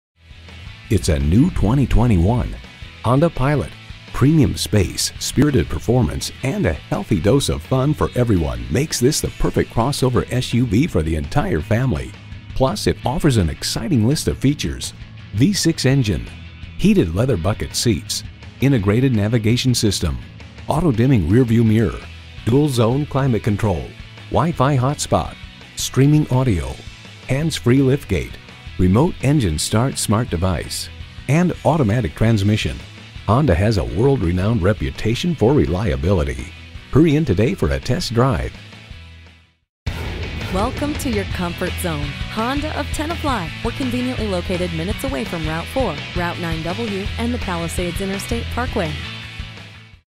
New 2021 Honda Pilot Teaneck Englewood, NJ #62355 - SOLD
Year: 2021
Make: Honda
Model: Pilot
Trim: Touring
Engine: 3.5L V6 24V SOHC i-VTEC
Transmission: 9-Speed Automatic
Color: Red
Interior: Beige
Stock #: 62355
VIN: 5FNYF6H68MB054196

Address:
28 County Road